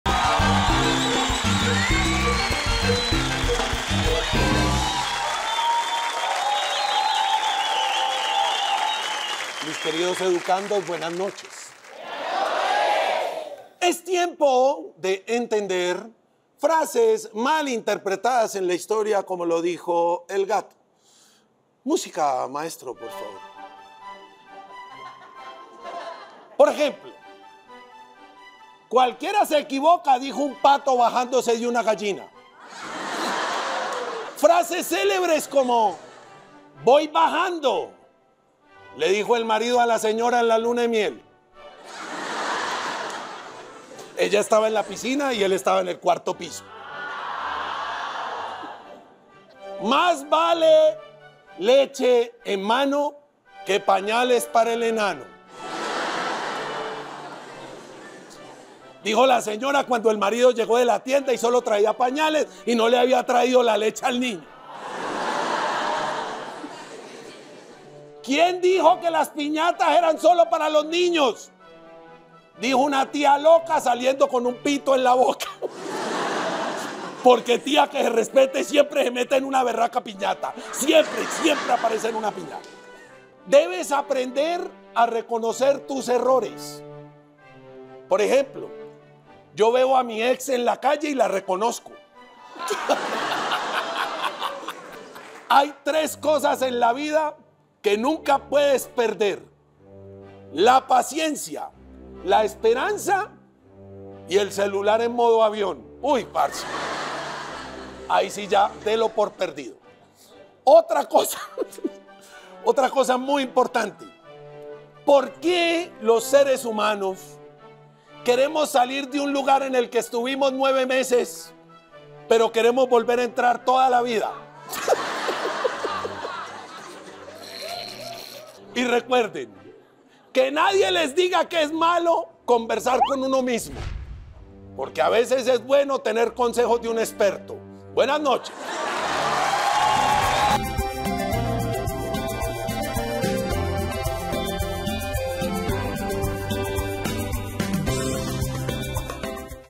El ‘Mono’ Sánchez nos da una clase para entender frases que se han malinterpretado
Con ejemplos muy graciosos, el humorista lee su lista de aquellas expresiones muy propias de los colombianos que usamos en la cotidianidad, pero que tienen un significado muy diferente.